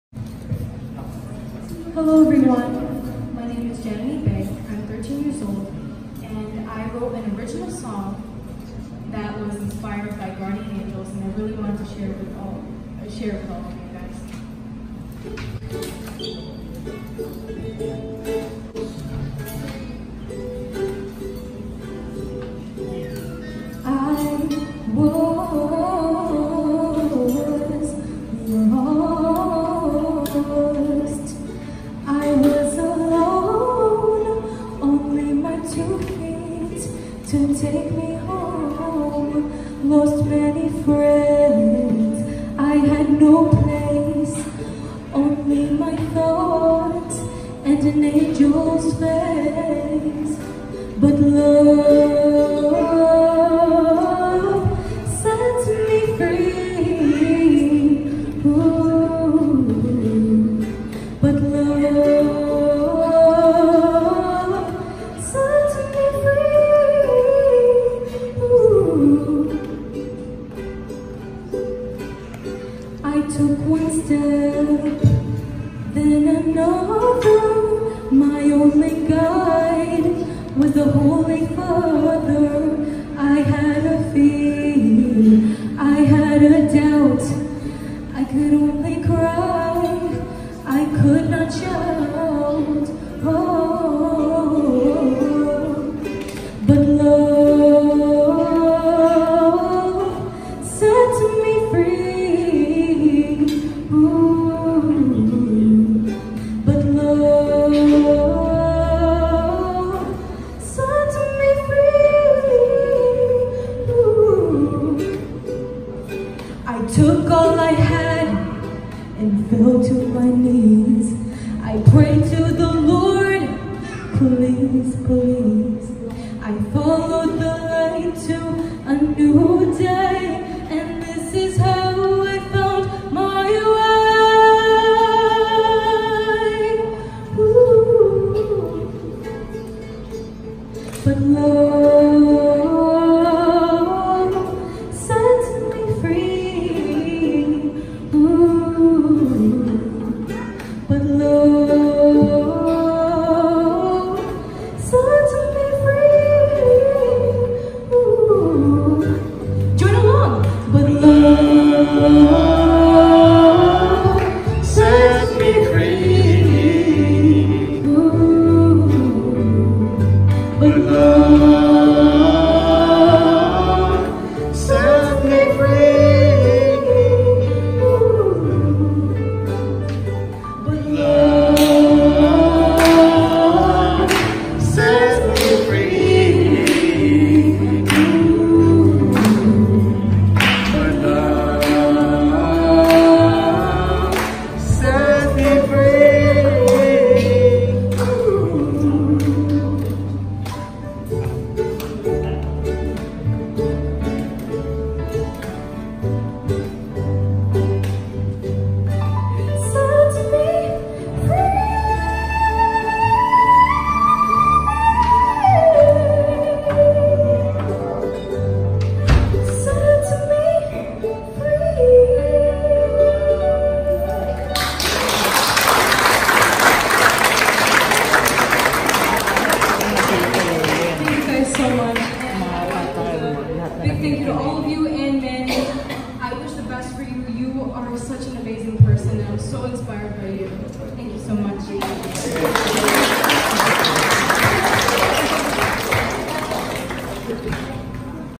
Middle Schooler sings really high note for Boxer Manny Pacquiao ( watch at end)
Sang during worship 😃